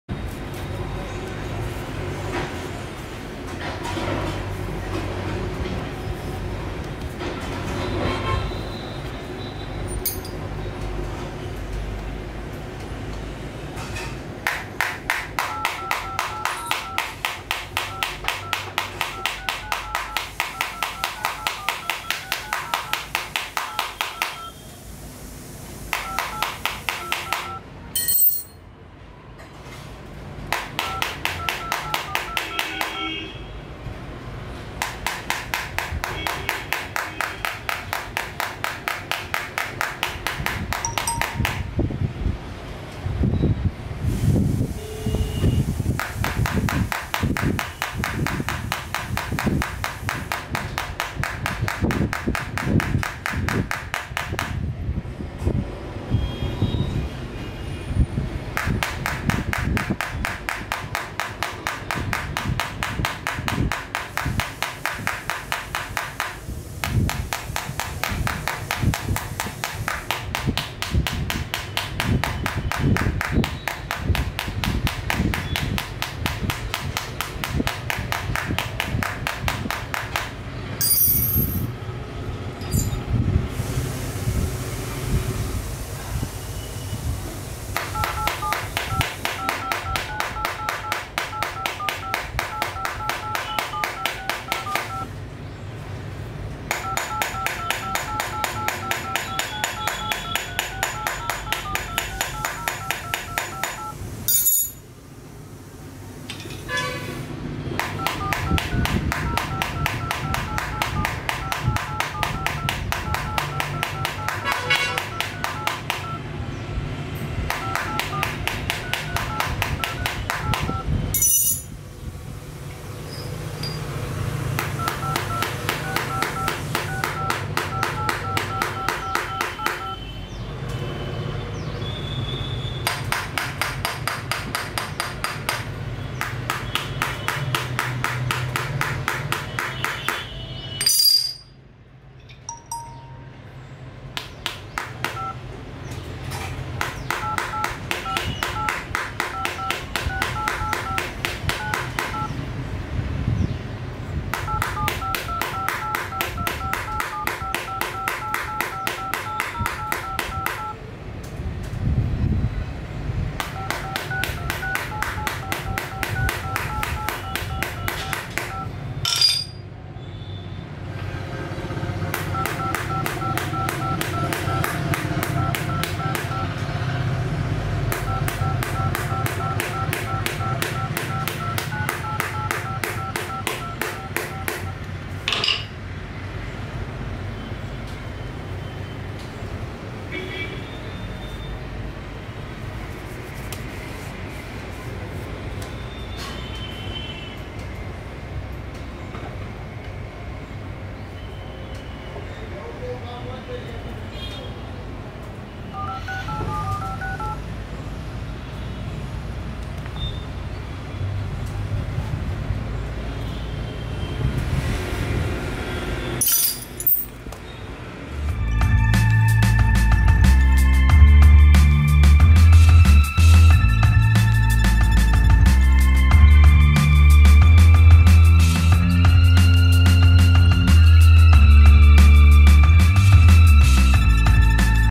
Nokia 2.1 Gorilla Glass Screen Scratch Test
Nokia 2.1 is comes with corning gorilla glass scratch proof display, and we test it.